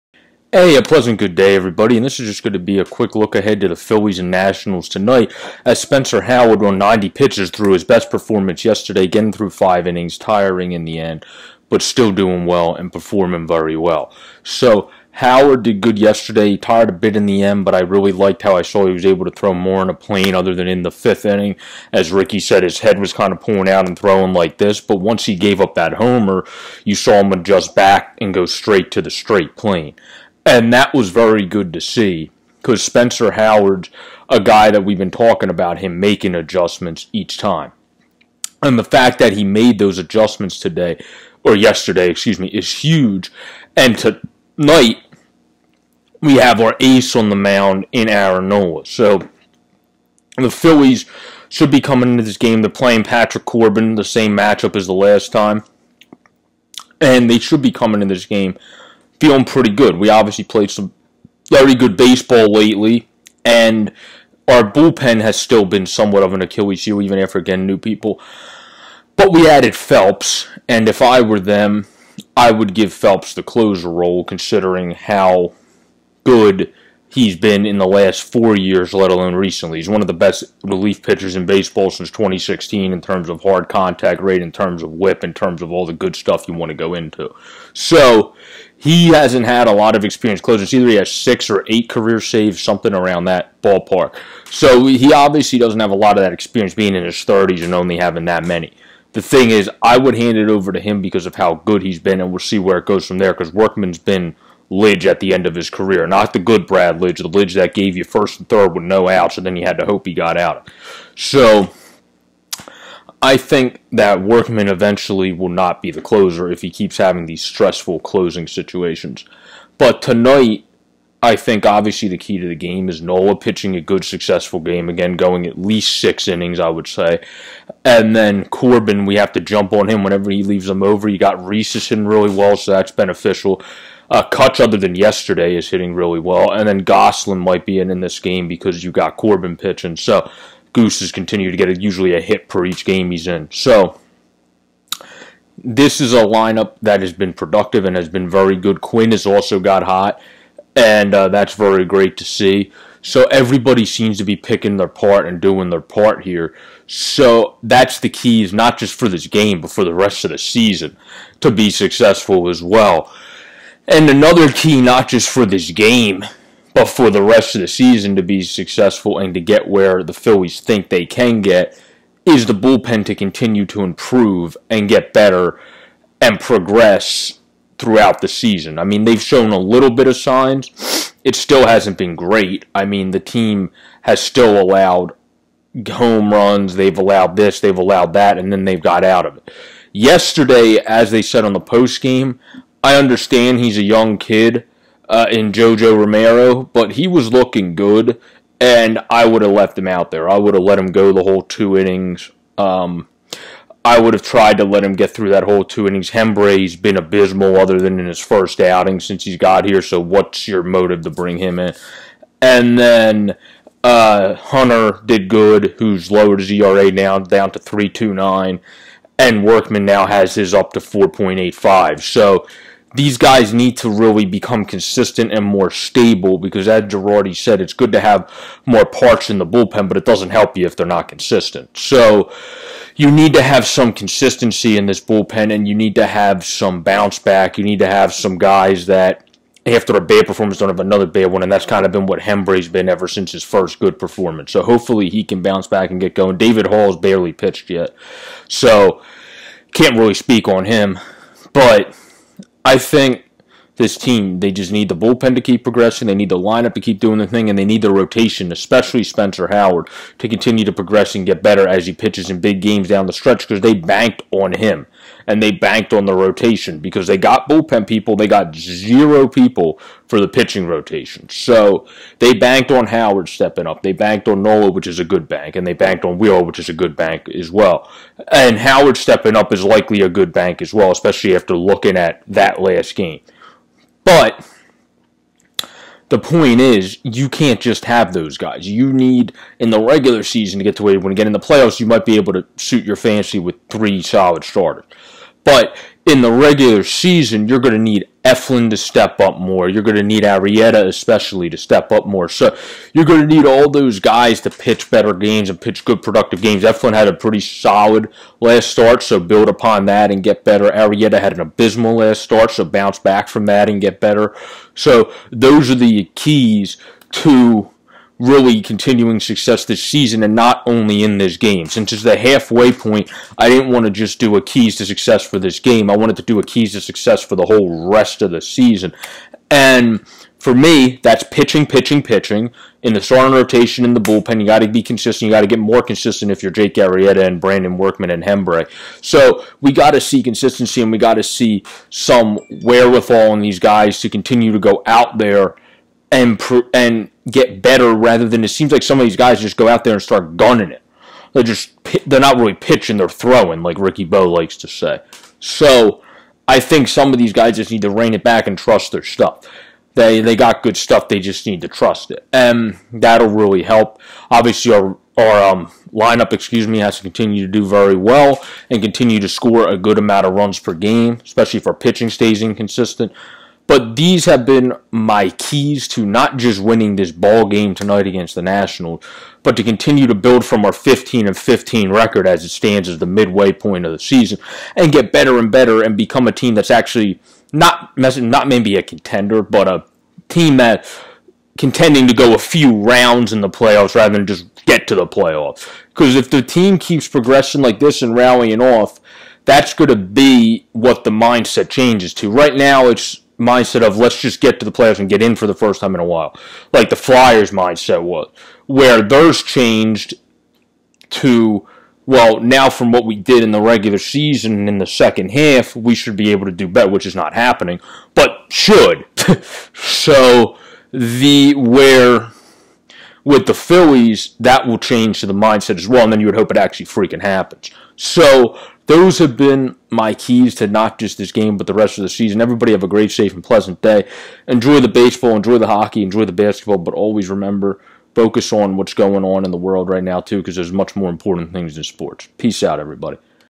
Phillies Keys to beating Nats and the Rest of the Season.
This is an episode all about Keys to the Game and Season as we are at the midway point. Yes, already the midway point of the Phillies Season. The Phillies look to build on their first half and grow as a team to become not just a playoff team. No, but a team that can actually make a statement at least in the first round.